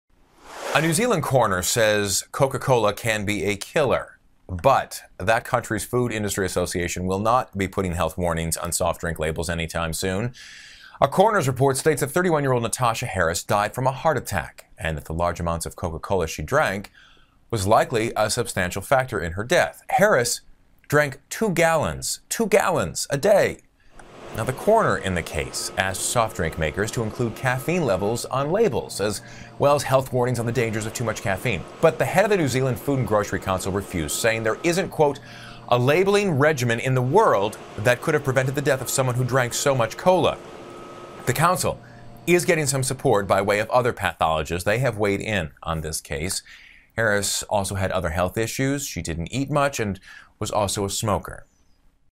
Coroner Links Mom's DEATH to Coke 'Addiction' | Coca Cola KILLED a Mother
WELLINGTON, New Zealand - A New Zealand food industry association on Wednesday rejected a coroner's call to add health warnings to soft-drink labels following the 2010 death of a woman who drank about eight litres of Coca-Cola a day.

Coroner David Crerar issued a final report Tuesday into the death of 31-year-old Natasha Harris, concluding that the mother of eight died from a heart attack. He said the large amounts of Coca-Cola she drank likely led to metabolic imbalances that gave rise to her heart problems, adding that Coke was likely a "substantial factor" in her death.

But New Zealand Food and Grocery Council chief executive Katherine Rich said "there isn't a labelling regime in the world" that could have prevented the death of somebody who chose to drink Coke in such large quantities.

The New Zealand branch of the Atlanta-based Coca-Cola Company, the world's largest beverage maker, disputed the coroner's findings, noting that experts could not agree on the most likely cause of Harris' death.

Crerar recommended that soft-drink makers consider including caffeine levels on the labels and warnings about the ill health effects if the drinks are consumed in excessive quantities.

Under New Zealand's food labelling system, warnings are mandatory on drinks with caffeine levels higher than 145 milligrams per kilogram. That covers many energy drinks, but not most soft drinks.

Rich said the coroner's recommendations were well-intentioned but ill-informed. She said that coffee, tea and chocolate also contain significant amounts of caffeine and that New Zealanders are unlikely to support the notion of health warnings every time they walk into a cafe or buy a chocolate bar.

She said the huge volume of Coca-Cola that Harris drank meant she couldn't receive adequate nutrients from other food sources.

"It doesn't matter what the food is. If it's consumed in excess, there will be ill health effects," Rich said. "It's clearly a very tragic and complex case, but not one that provides a strong argument for changes to labelling."

At a 2011 inquest into Harris' death, Vivienne Hodgkinson, the mother of Harris' boyfriend, said that Harris always needed to have Coca-Cola available, and that if she ran out she would "get the shakes, withdrawal symptoms; be angry, on edge and snappy."

The coroner also heard evidence that Harris was of normal weight, didn't eat much or drink any alcohol, and smoked about 30 cigarettes a day. She drank only regular Coke, preferring the taste over diet or caffeine-free varieties. The coroner heard that Harris had no energy in the months leading up to her death, felt ill all the time and often vomited and experienced a racing heart.

The coroner said it was hard to be certain about how much Coke Harris drank, but after reviewing evidence from her partner and friends, as well as 51 supermarket receipts, he estimated it was between six and 10 litres per day.

The pathologist who examined Harris found that she likely suffered from hypokalemia, or low potassium, which contributed to her health problems, and that she had an enlarged liver from excessive sugar consumption.

Another pathologist giving expert testimony said that excessive cola consumption "can be dramatically symptomatic, and there are strong hypothetical grounds for this becoming fatal in individual cases."

But a third pathologist giving evidence for Coca-Cola said that Harris' health problems could have been caused by anemia or diabetes, and that genetic factors may also have played a role.

In a statement, Coca-Cola Oceania said it was disappointed the coroner chose to focus on "the combination of Ms. Harris' excessive consumption of Coca-Cola, together with other health and lifestyle factors, as the probable cause of her death. This is contrary to the evidence that showed the experts could not agree on the most likely cause."